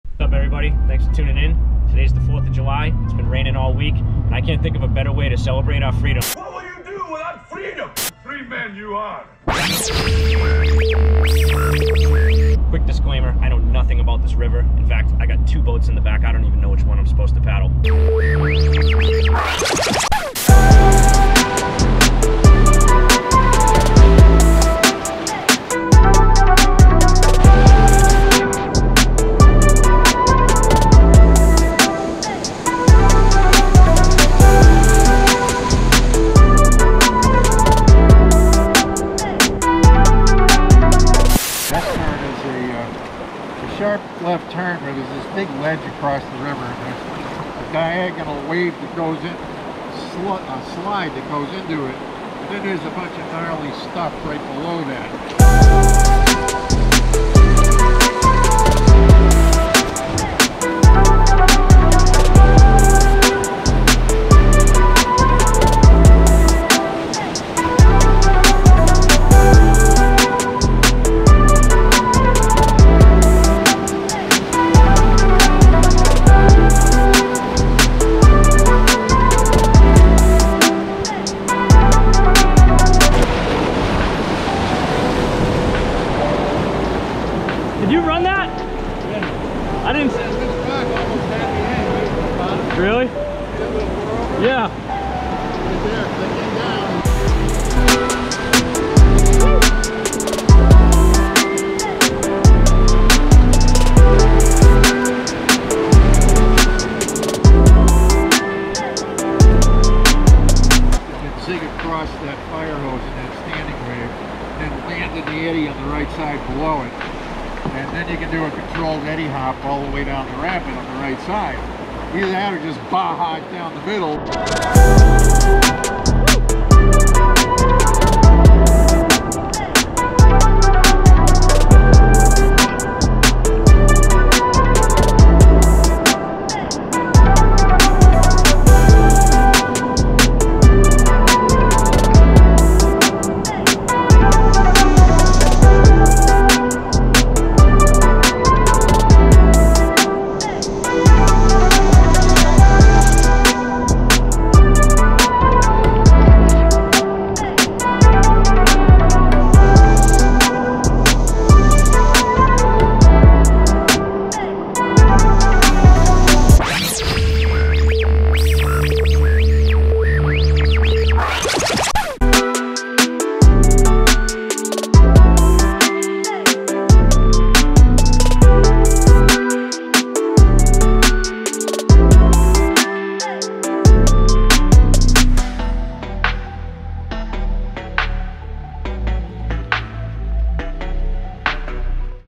WHITEWATER SESSIONS Ep. 2 [CONTOOCOOK]
Take a lap down the Contoocook with me on the 4th of July in Henniker NH.